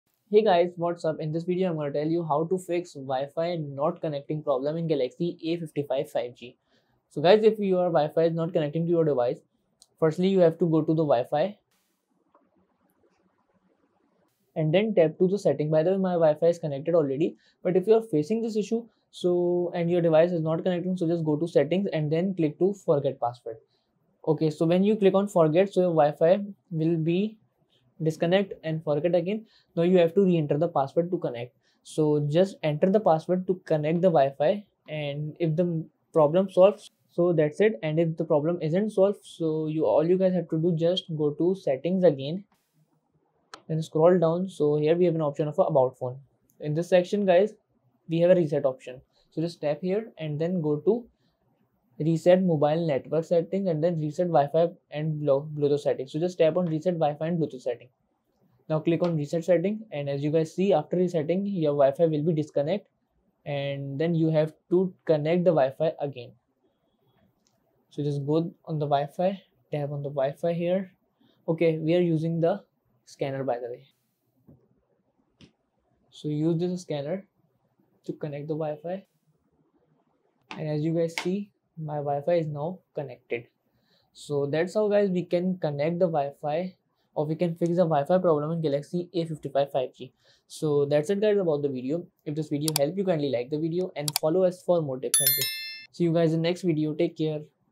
Samsung A55 5G : FIX Wifi Not Connecting
How to fix wifi not connecting issue in Samsung A55. To solve wifi issues or restore default wifi settings in Samsung A55 5G, follow this steps.